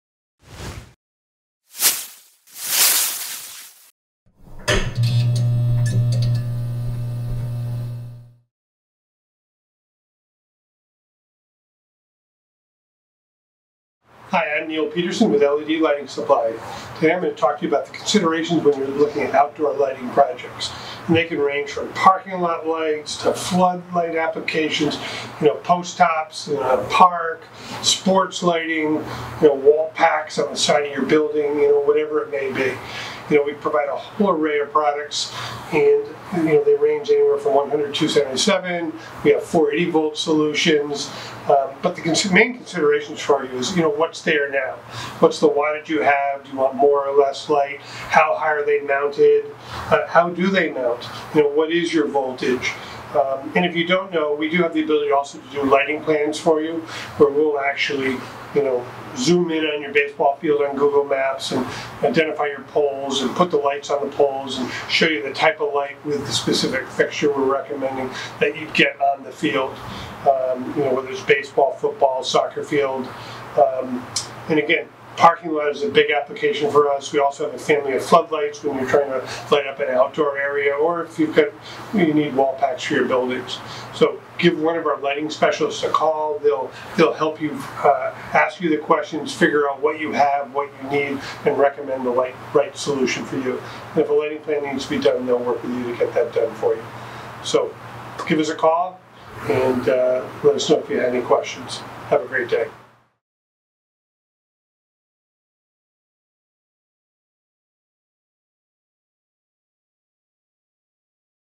Expert Tips: How to Choose the Best Outdoor Lighting Fixtures
In this video, Neil Peterson of LED Lighting Supply takes you through the decision-making process when it comes to choosing the best lighting fixtures for your outdoor projects from sports fields, to parking lots, and everywhere in between.

Featured Lighting Fixtures:
- 90 / 120 / 150 Watt Adjustable LED Flood Light (MLLG-LED-FLD5-150-CC-PC
- 80 / 100 / 120 Watt LED Decorative Post Top Fixture ( MLLG-YA-LED-PTD-120-CC)
- 400 Watt LED Stadium Light | 60000 Lumens (MLLG-LED-HPSL-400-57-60-[F])
- 90 Watt LED Wall Pack (MLLG-YA-LED-WPSC-90-50)
- 90 / 120 / 150 Watt Adjustable LED Shoebox Area Light (LLS-SBHO4-90/120/150-CC)

Transcript:

Hi, I'm Neil Peterson with LED Lighting Supply, today I'm going to talk to you about the considerations when you're looking at outdoor lighting projects and they can range from parking lot lights to flood light applications you know post tops, parks, sports lighting, you know, wall packs on the side of your building you know, whatever it may be, you know we provide a whole array of products and you know they range anywhere from 100-277.

We have 480-volt solutions but the main considerations for you are, you know, what's there now, what's the wattage you have, do you want more or less light, how high are they mounted, how do they mount. You know what, is your voltage and if you don't know, we do have the ability also to do lighting plans for you where we'll actually, you know, zoom in on your baseball field on Google Maps and identify your poles and put the lights on the poles and show you the type of light with the specific fixture we're recommending that you'd get on the field, you know whether it's baseball, football, soccer field and again parking lot is a big application for us.

We also have a family of flood lights when you're trying to light up in an outdoor area or if you've got you need wall packs for your buildings so give one of our Lighting Specialists a call they'll help you, and ask you the questions, figure out what you have, what you need and recommend the light right solution for you and if a lighting plan needs to be done they'll work with you to get that done for you so give us a call and let us know if you have any questions. Have a great day!